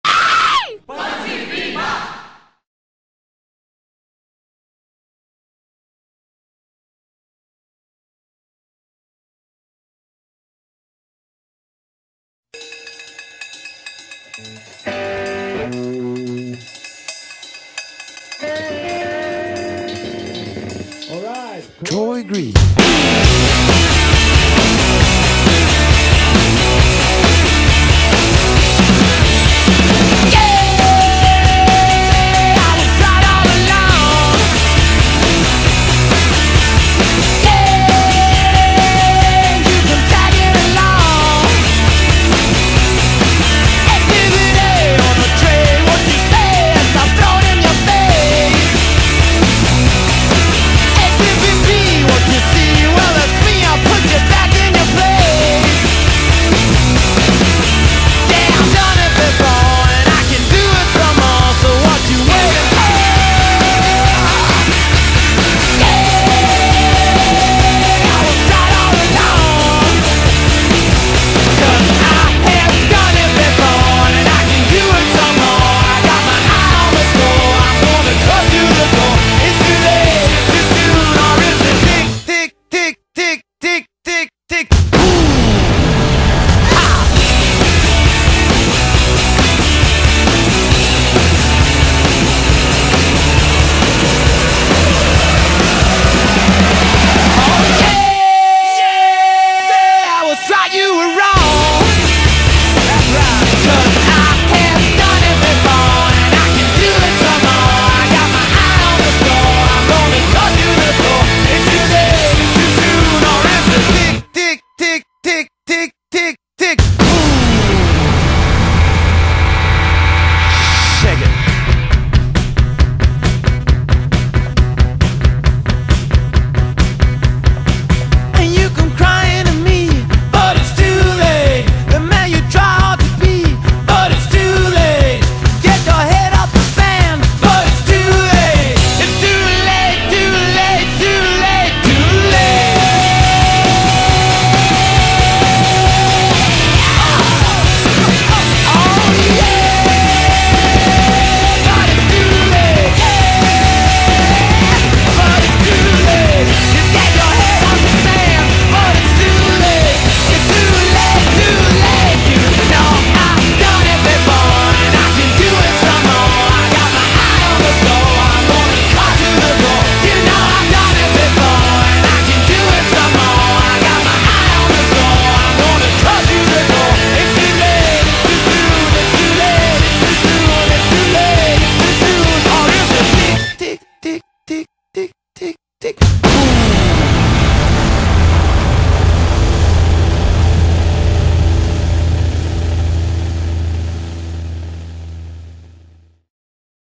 Operação SOFRE BIXO - Comunicação Social UFC 2009.2
A semana de recepção dos bix@s de Comunicação Social UFC foi massa! Teve de tudo: tchutchu em veterano, a prova da banana, revelações no Bix@s on the table, loura do banheiro, patinho, elefantinho...

Esse vídeo é uma homenagem pra galera que participou dessa semana inesquecível sofrendo ou fazendo sofrer!